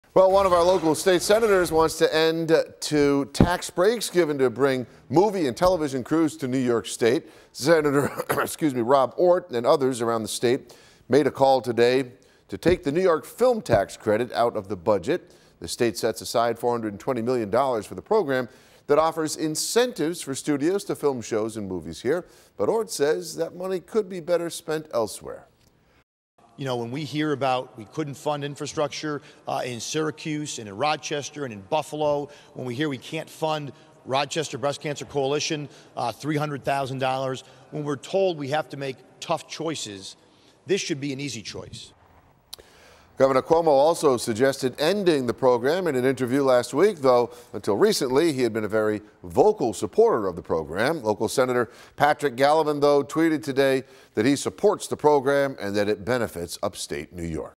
Ortt Wants End To State Film Tax Credit
Ortt sy money could be better-spent elsewhere